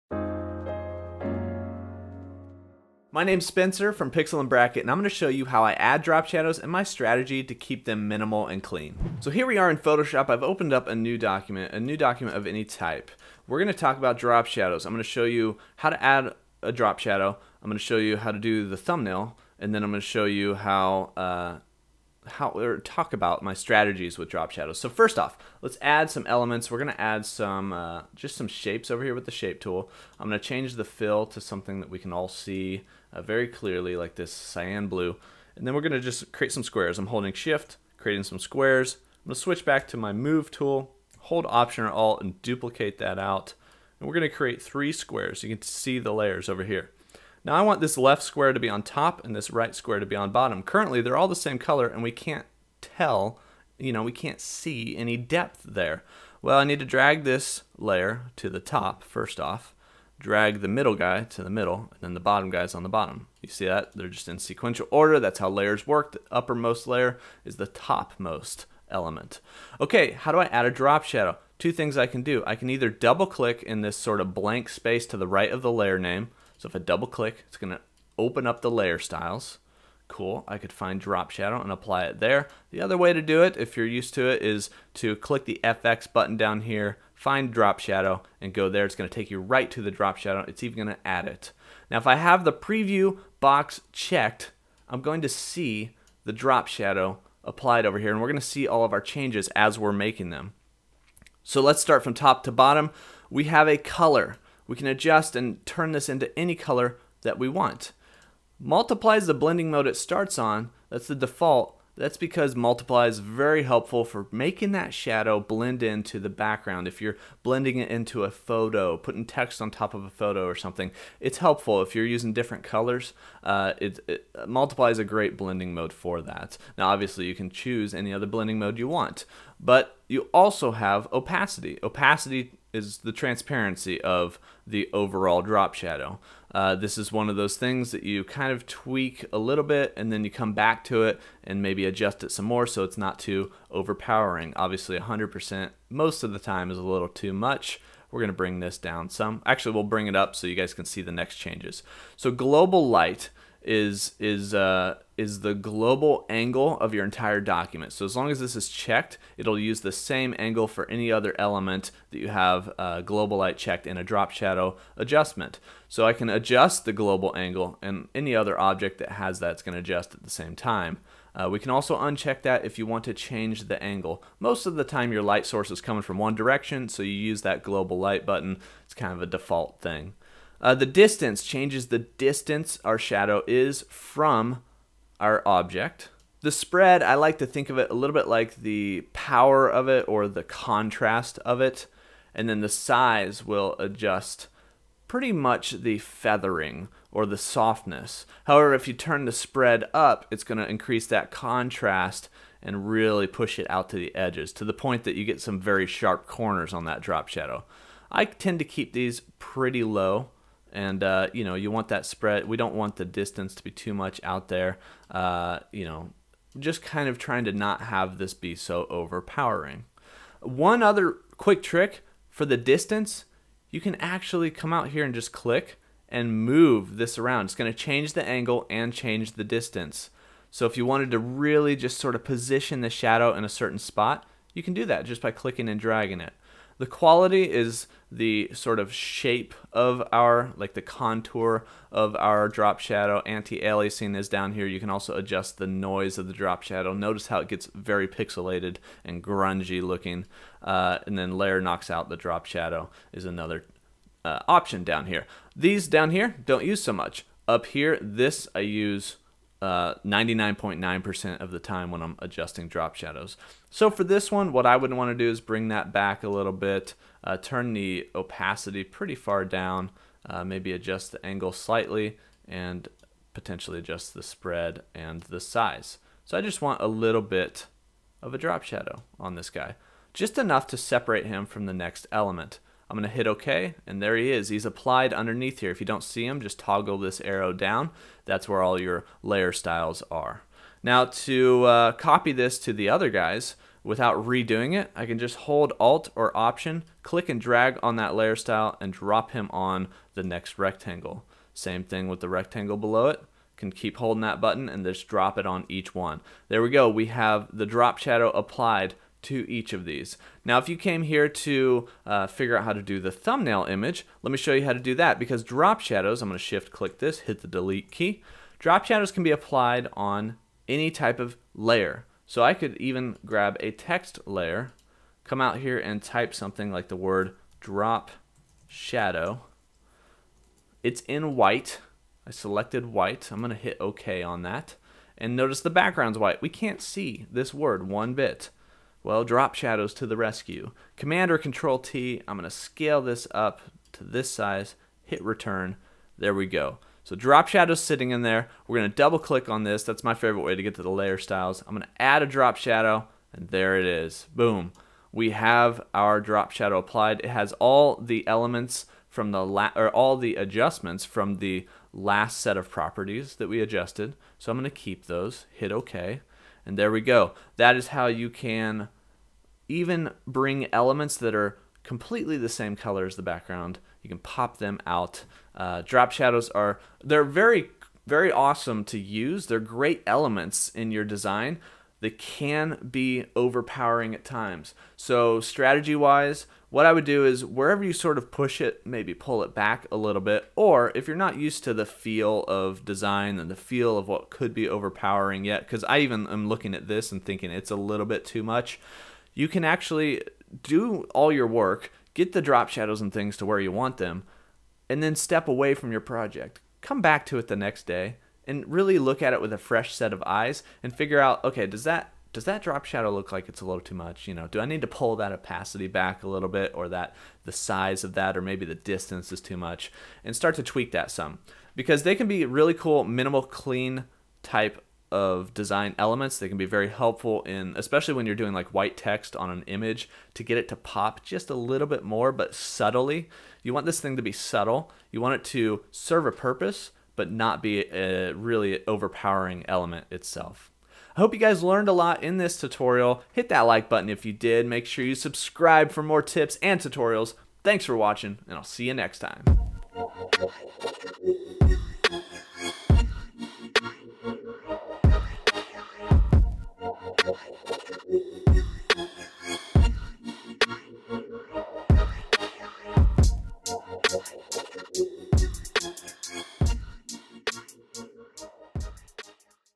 How to Add Drop Shadow | Photoshop CC Tutorial
In this Photoshop tutorial, learn how to add drop shadow to text and layers. It can be found in layer styles under drop shadow. I also talk about my strategies to keep drop shadows from overpowering your design.